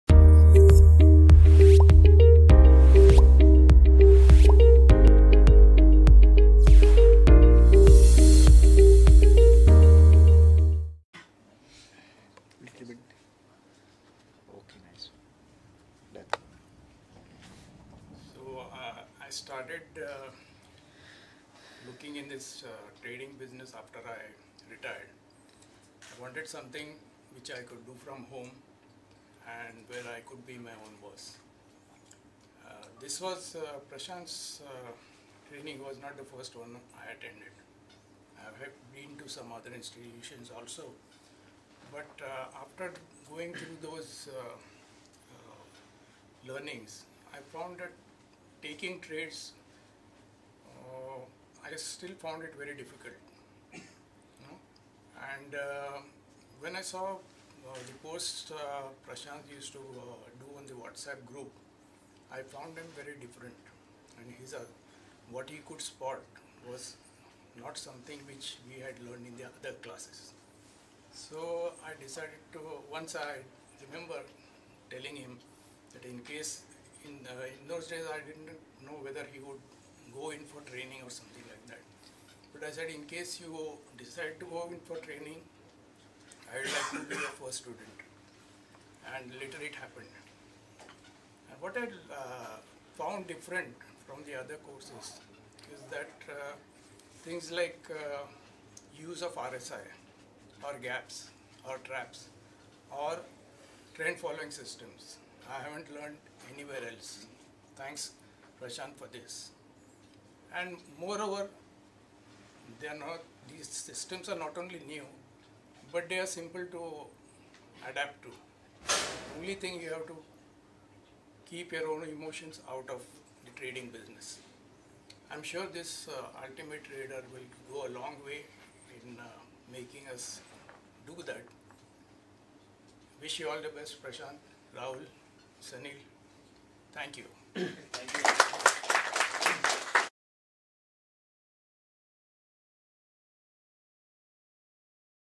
Catalyst Wealth - Student Testimonial - Girish Gokhale
Mr. Girish Gokhale is a Pharmacist by profession. After retirement, he wanted to do something from his home independently which not only would keep him busy but also give him some income. Trading offered this opportunity. Hence after retirement, he went for training in this field. At Catalyst Wealth, he came across with the right knowledge of Technical Analysis that provided him with the freedom to trade online.

Before that, his dealing with the share market were limited to investing a little during the period when many multinationals had to dilute their shareholding by public offers to Indian investors. Never traded as such during that time.